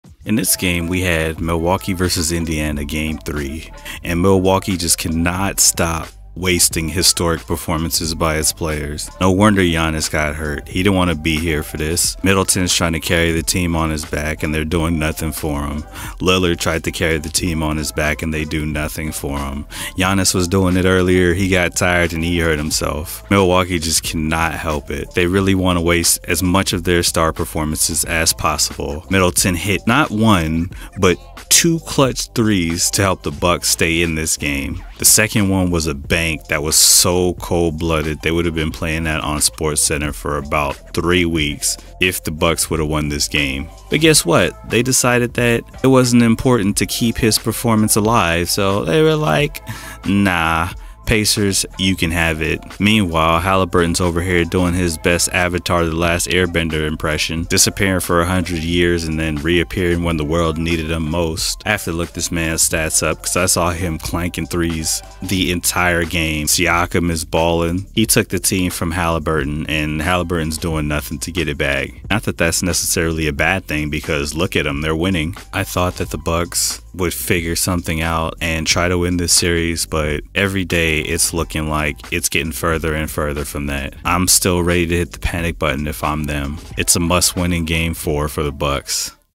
Quick Hits: The Bucks Can't Stop Wasting Clutch Performances
@drdondondoggy on all social media platforms

Game 3: Indiana Pacers vs. Milwaukee Bucks

It still might be panic time for the Bucks